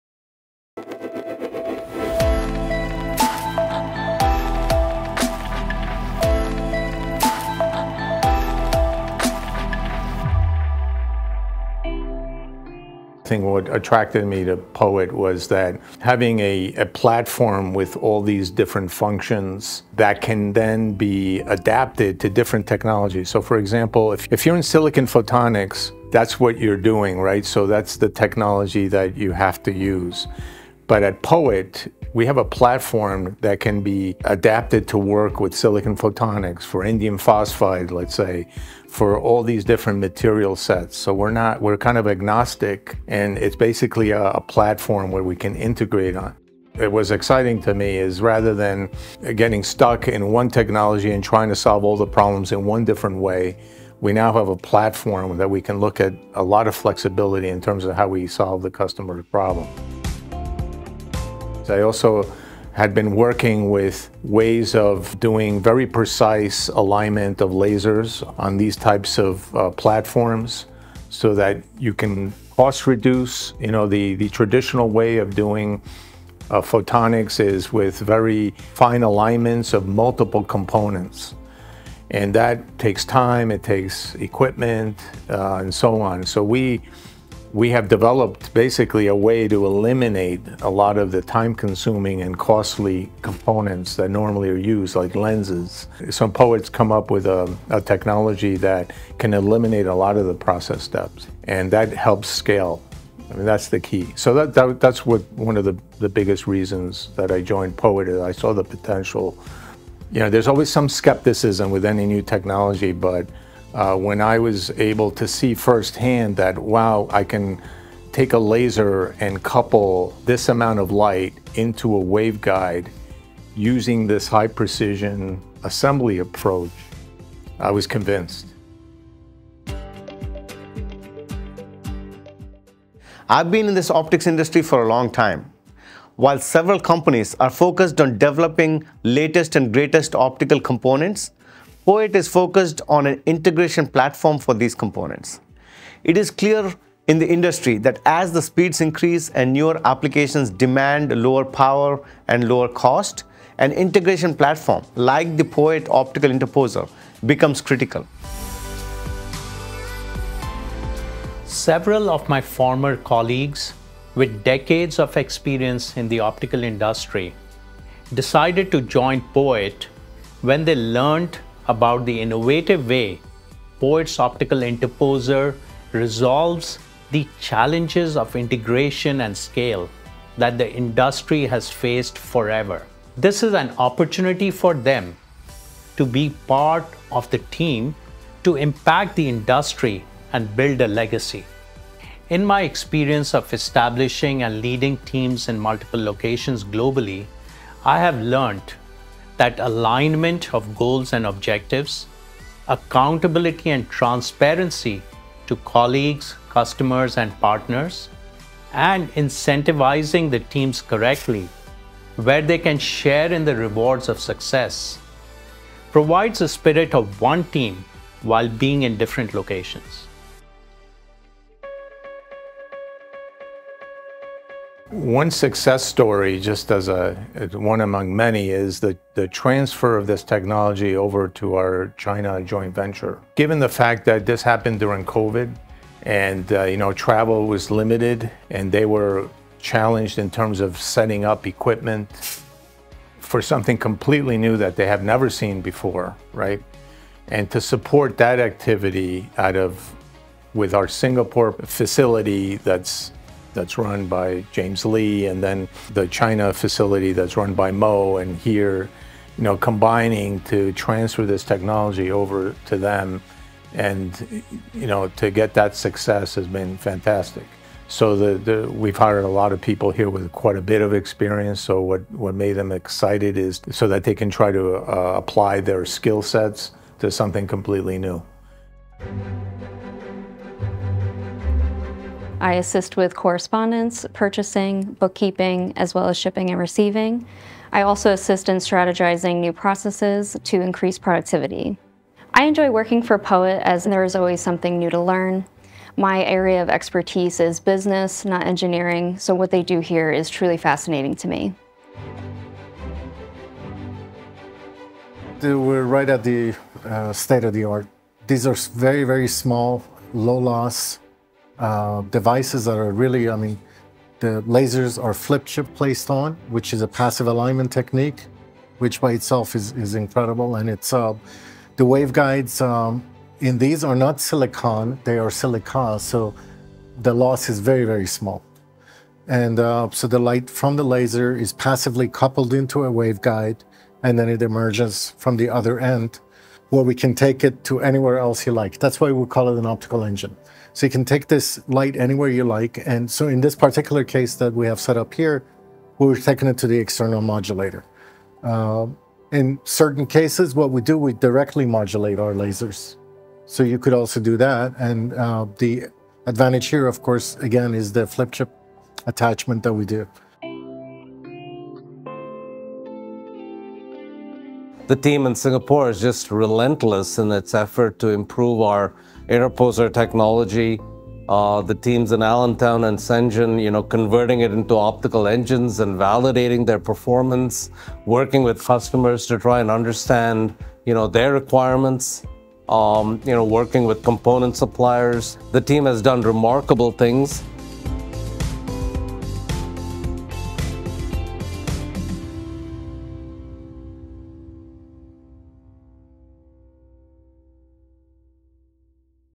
Building a Legacy in Photonics at POET Technologies
Go behind the scenes with the POET Technologies team as they work on photonics innovations that are changing the AI, datacom, and telecom industries.